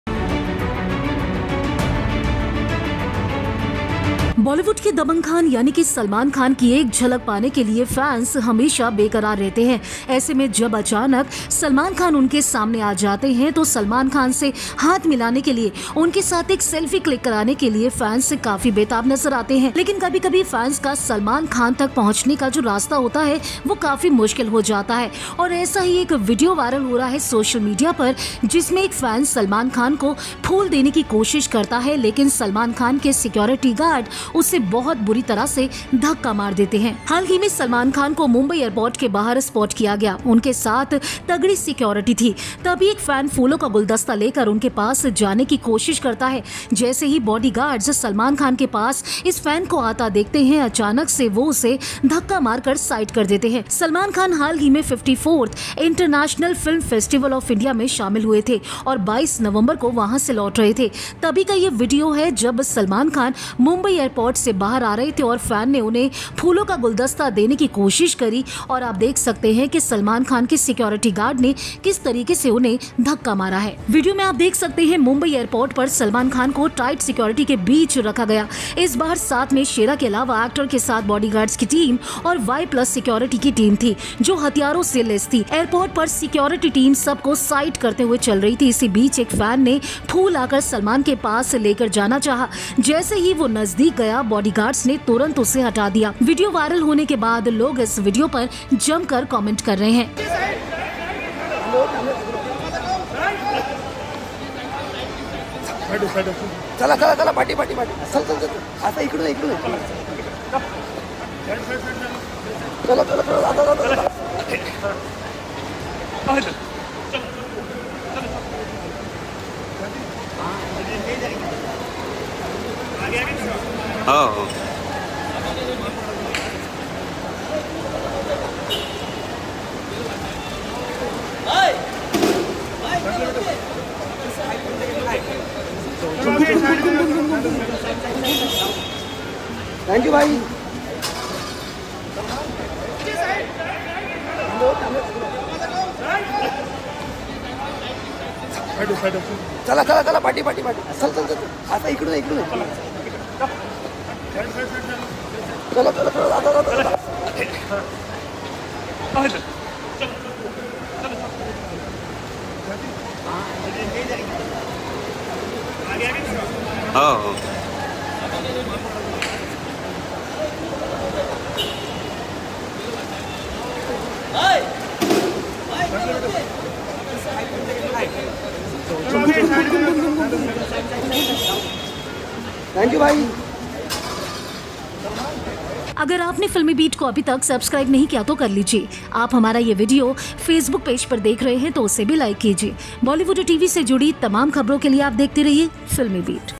Salman Khan की तगड़ी Security में घुसा Fan तो Bodyguard ने मारा धक्का, Actor को देने जा रहा था फूल
Salman Khan's team of bodyguards pushed a man aside after he breached security to get closer to the actor. A video of the same is now going viral. Watch Out